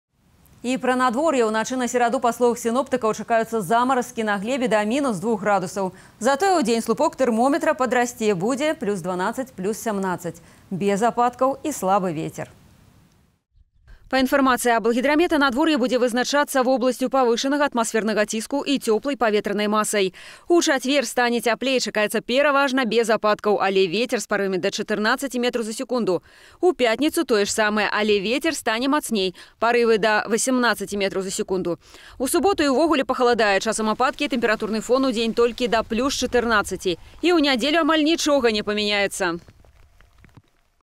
Погода на Могилевщине 09.10.2024 // Облачно, без осадков
По словам синоптиков, в среду, будет облачно и преимущественно без осадков. Ветер слабый. Ночью возможны первые заморозки. Температура воздуха ночью 0...+2 °С, днем +12...+17 °С.